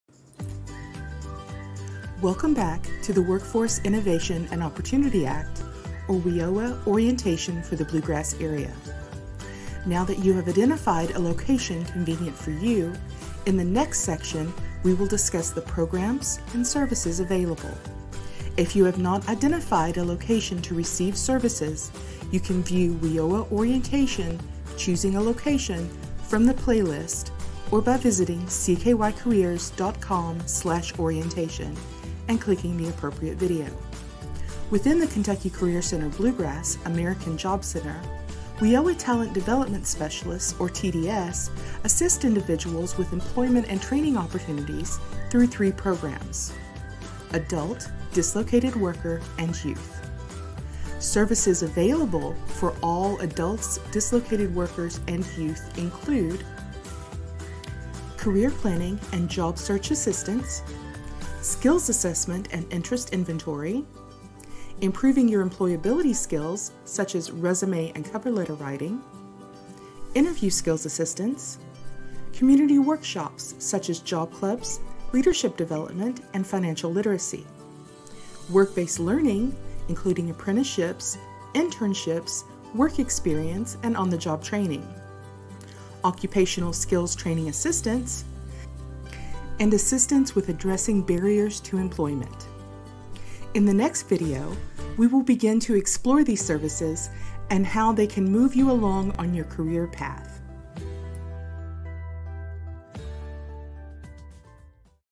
WIOA Orientation: Part 2 (Services)
Within the Kentucky Career Center – Bluegrass/American Job Center, WIOA Talent Development Specialists or TDS assist individuals with employment and training opportunities through three programs: Adult, Dislocated Worker, and Youth.
Services available for all adults, dislocated workers, and youth include:
• Career Planning & Job search assistance
• Skills assessments & Interest inventory
• Improving your employability skills such as:
o resume and cover letter writing
o interview skills assistance
• Community workshops, such as Job Clubs, Leadership Development, and Financial Literacy
• Work-based Learning: (Examples include Apprenticeships, Internships, Work Experience, and On-the-Job Training)
• Occupational Skills Training Assistance
• Assistance with addressing barriers to employment.